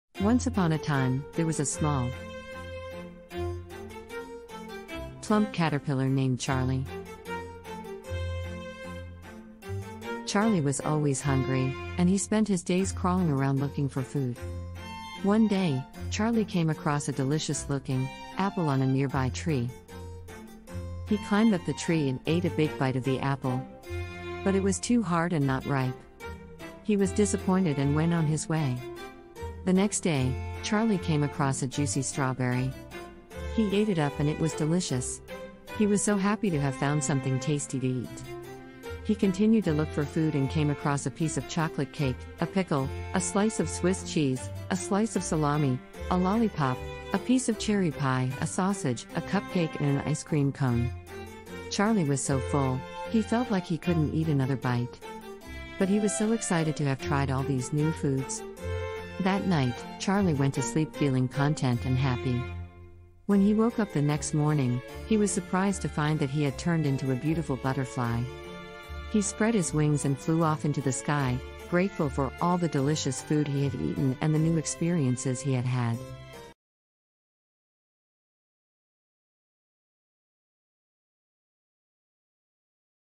Charlie the Curious Caterpillar: A Tasty Adventure
A chubby, happy, human baby-faced green caterpillar goes on a tasty adventure as it tries different foods like a juicy strawberry, a piece of chocolate cake, a pickle, a slice of Swiss cheese, a slice of salami, a lollipop, a piece of cherry pie, a sausage, a cupcake, and an ice-cream cone. Watch as the caterpillar's taste buds are tantalized and its belly is filled with delicious treats. See the caterpillar's expressions change as it tries new foods and ultimately transforms into a beautiful butterfly. Join us on this fun and educational journey as we discover the world of food through the eyes of a hungry caterpillar. Perfect for children of all ages!